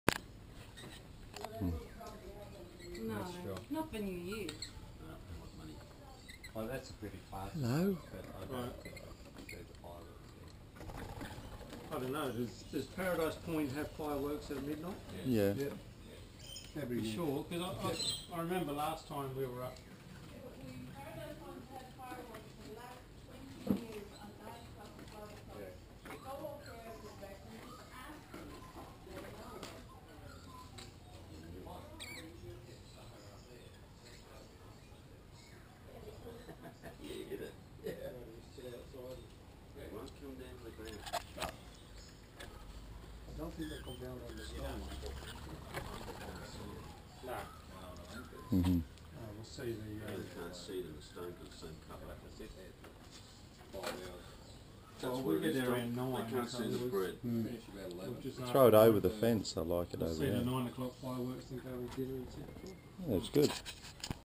COOMBABAH PARROTS IN BETTY AND TOMS BACKYARD
Enjoy the tame parrots on the Gold Coast. They visit Tom and Betty Harrisons place every morning for a feed.